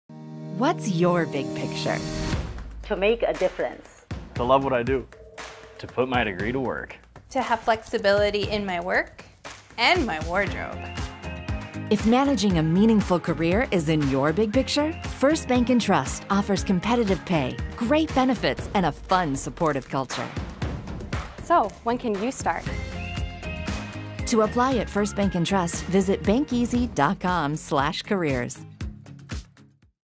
First Bank & Trust Big Picture Recruitment
StoryBuilt was asked to create a fun, lighthearted recruitment video for a local bank. This was one of five TV spots created for the client. Great script writing and good visuals led to a very successful campaign.